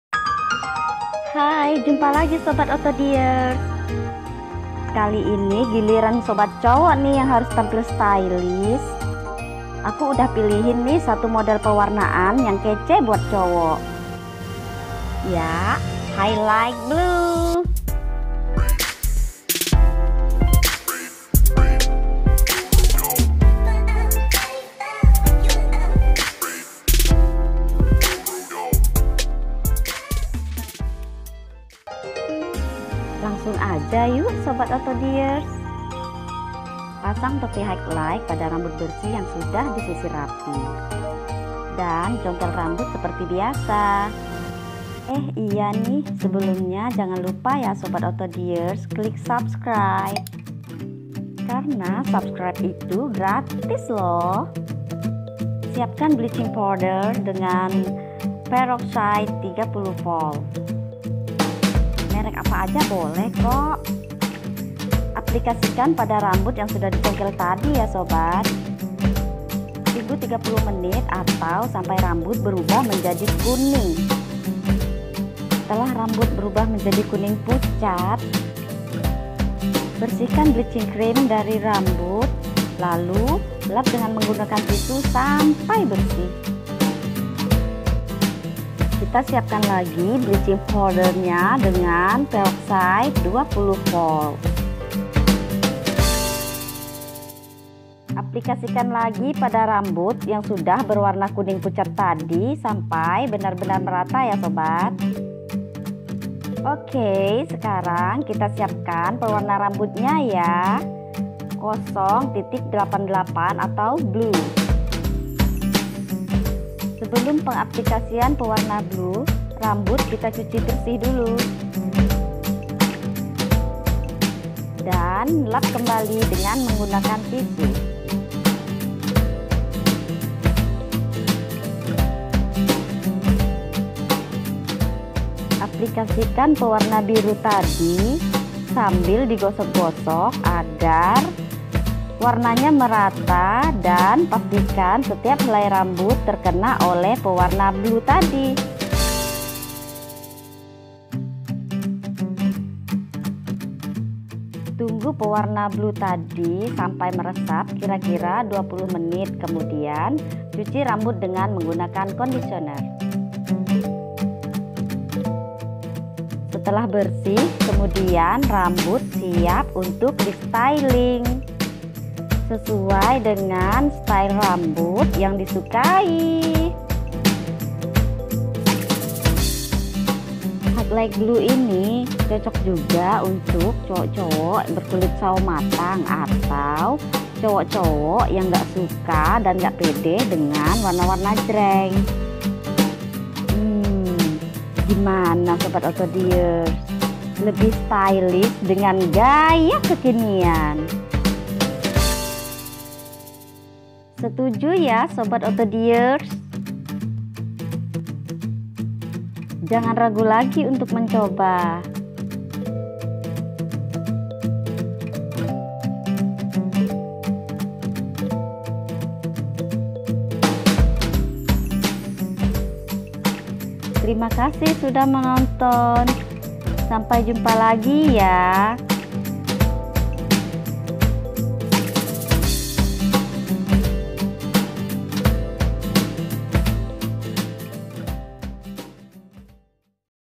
trend warna rambut cowok#HIGHTLIGHT_BLUE untuk kulit gelap
saat ini bukan hanya kaum wanita saja yang butuh gaya, khusus nya gaya rambut.
trend warna rambut saat ini makin di gandrungi baik oleh wanita maupun pria.
salah saru trend warna rambut yang cocok untuk pria berkulit gelap adalah tehnik pewarnaan rambut hightlight blue.
warna blue/biru adalah salah satu warna maskulin yg sangat cocok d gunakan oleh pria namun sangat cocok pula d gunakan oleh wanita.